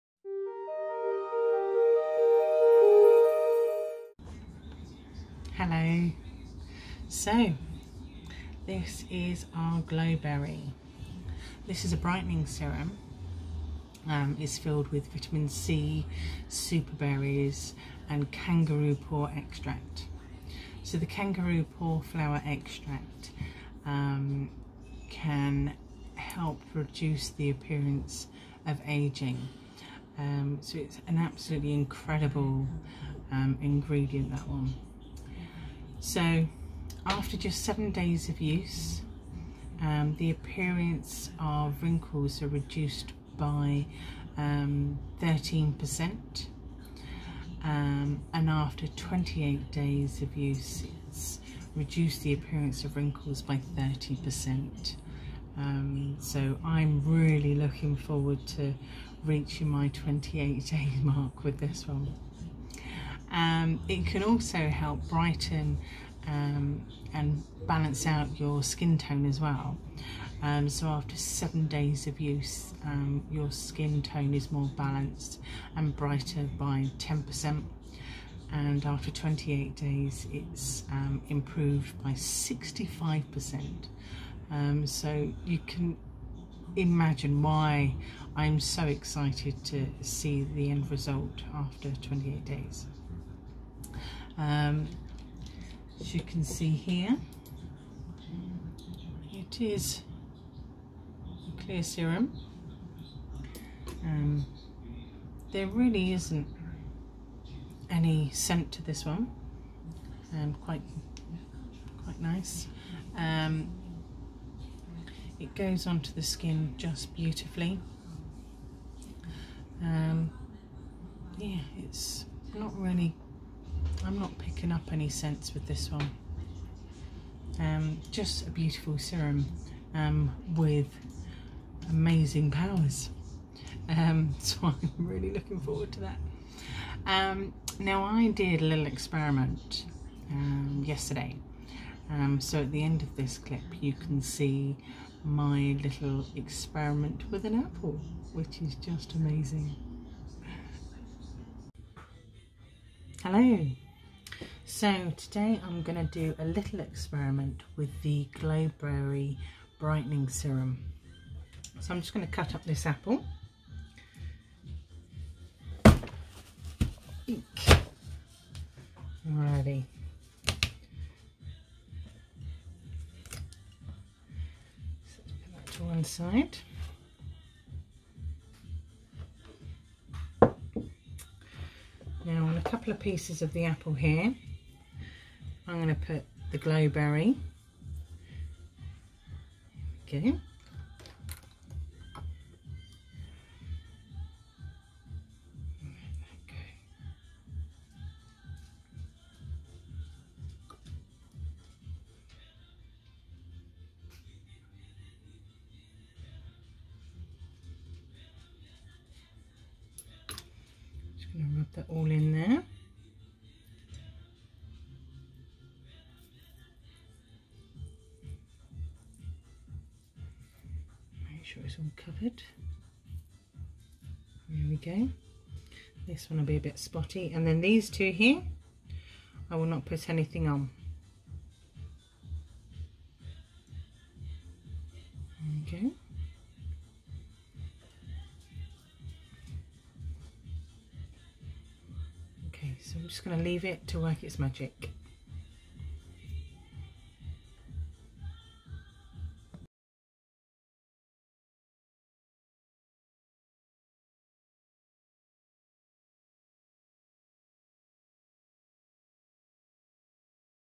Glow Berry Experiment - Tropic Skincare with Sabrina 💋
Tropic Skincare with Sabrina 💋 Glow Berry Experiment

A powerful blend of super-brightening berries and extracts to reduce hyperpigmentation and give you radiant, younger-looking skin

Please note the review and comments that are made are my views and not that of Tropic Skincare

If you wish to discover more, drop me a message 💋